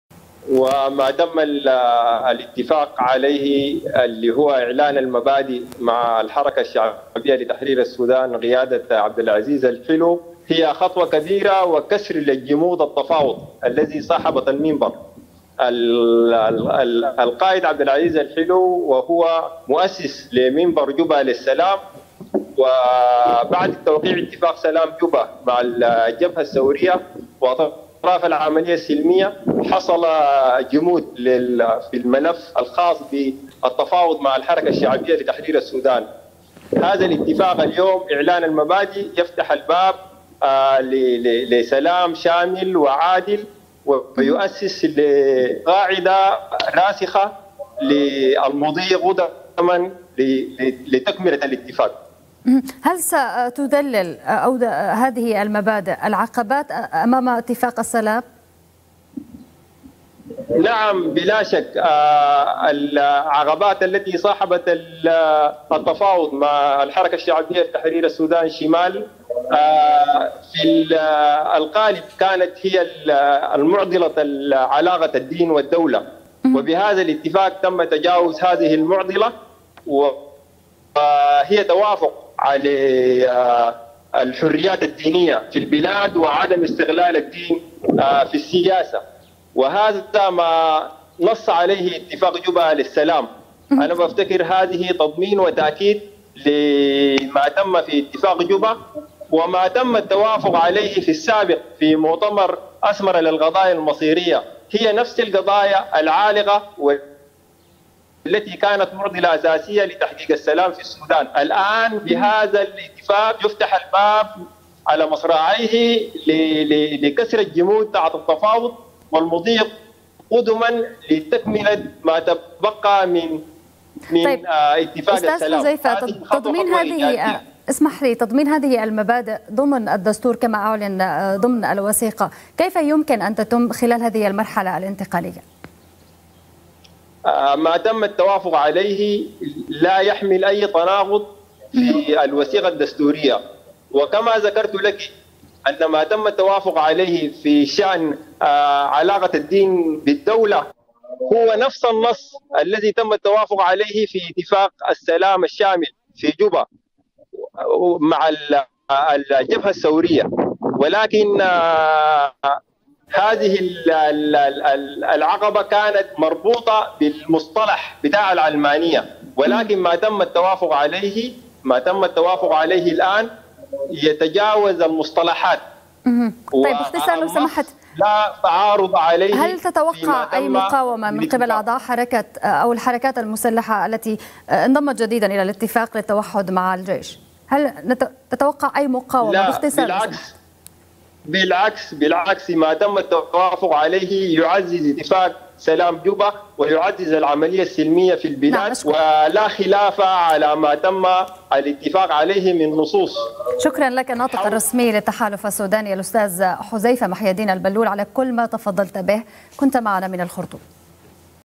حذيفة محي الدين البلول: إعلان المبادئ مع الحركة الشعبية -شمال- يفتح الباب لسلام عادل وشامل ...
الخرطوم
حذيفة محي الدين البلول ـ الناطق الرسمي للتحالف السوداني: إعلان المبادئ مع الحركة الشعبية -شمال- يفتح الباب لسلام عادل وشامل في السودان، وماتم التوافق عليه يتجاوز المصطلحات ويستكمل اتفاق جوبا.

تابعوا قناة اليوم على التردد:
 12687_27500 (H) _NILESAT